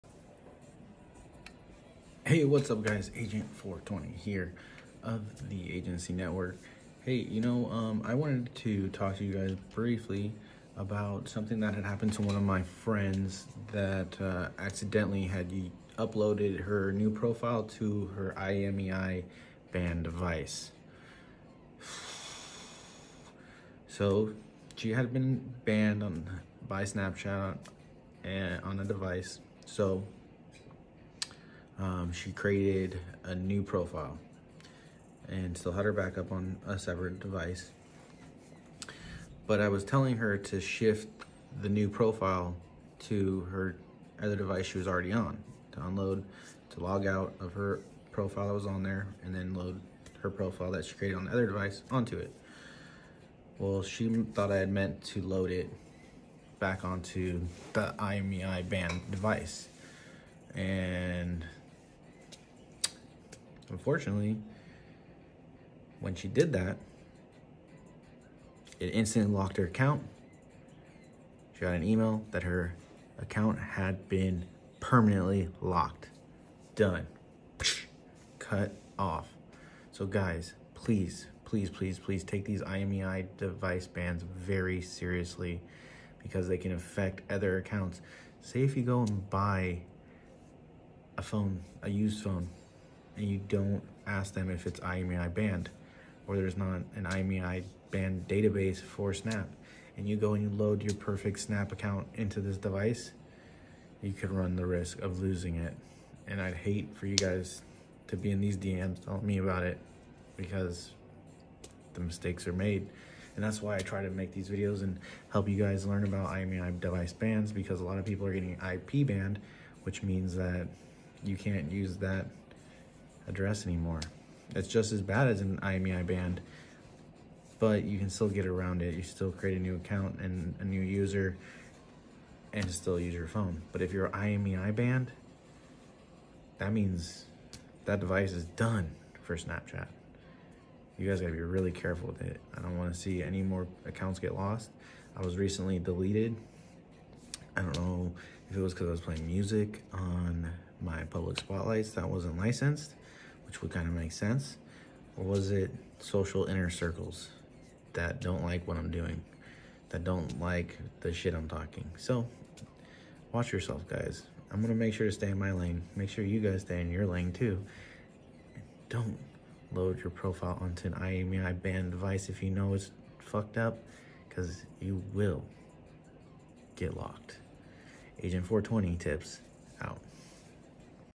DONT use IMEI BANNed DEVICES
Hey guys if you use a IMEI Banned phones with a new account. you could run the risk of losing your new account permanently!! so make sure you ask when buying a new used phone!!! if it’s been banned!! if you load these a profiles onto it they will be locked permanently! good luck out!! there stay safe￼ and don’t get jacked!!